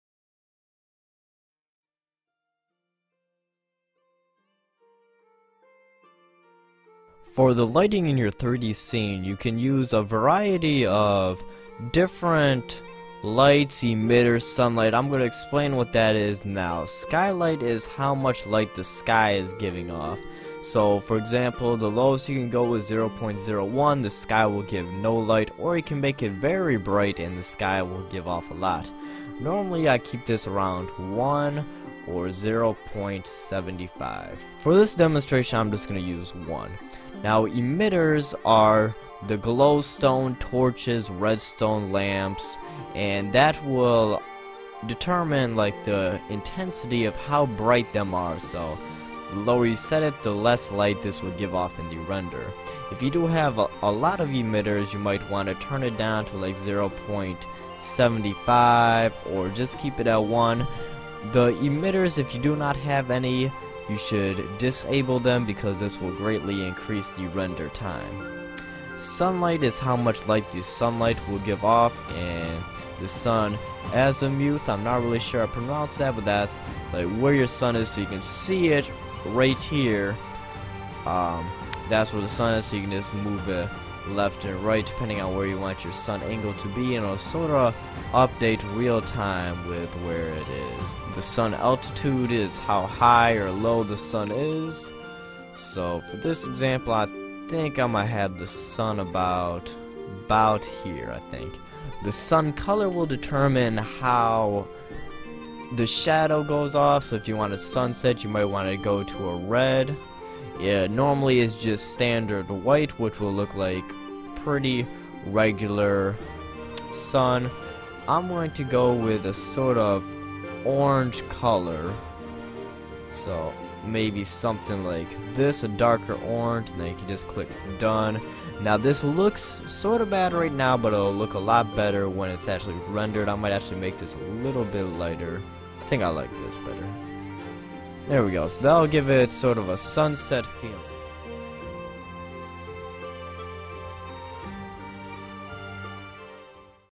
Minecraft Chunky Tutorial - Lighting
In this tutorial series, I will be showing you many different aspects of Chunky, an application used to create detailed renders of Minecraft worlds and maps. I will guide you through from start to finish, as we go about learning how to use Chunky.